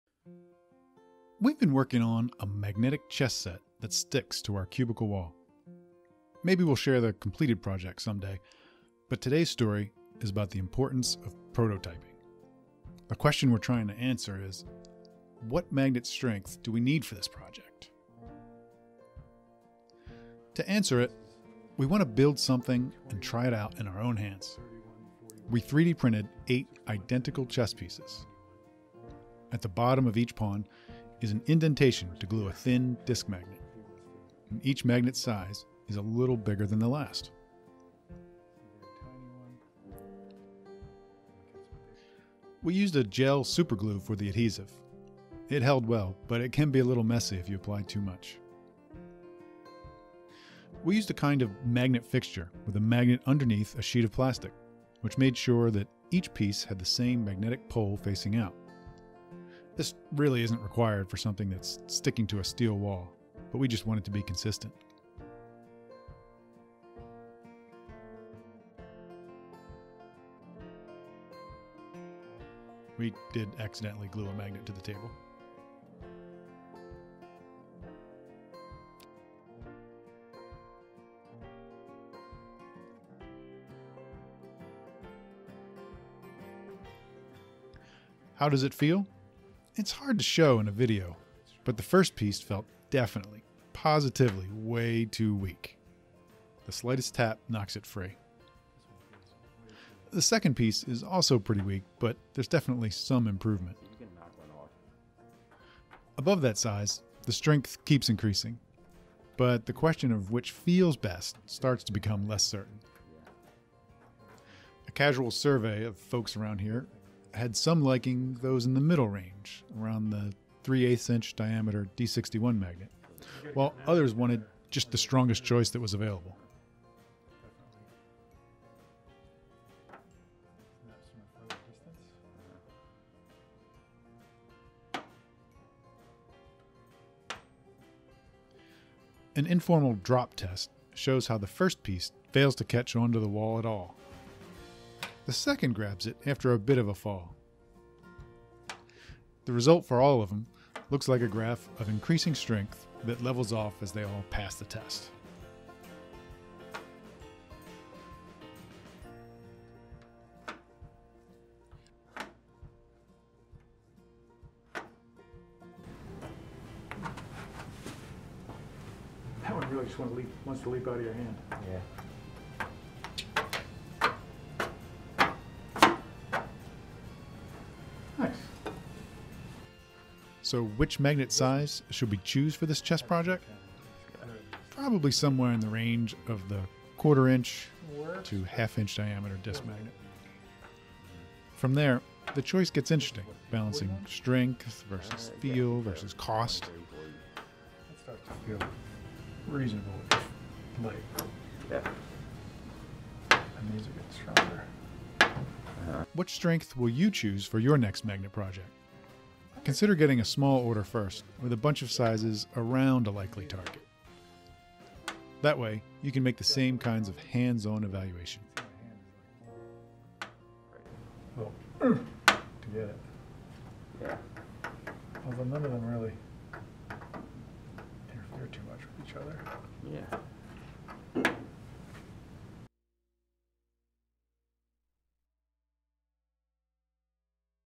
Magnetic Chess Board - Prototype!
We’re not making a complete magnetic chess set today.  Though that’s something a few of us always seem to be tinkering with, this article is more about an important piece of the process: Prototyping.

How much pull force do I need for a magnetic chess piece?  What magnets will provide that force?

From experiments, to DIY projects, to smash videos, we've got it all!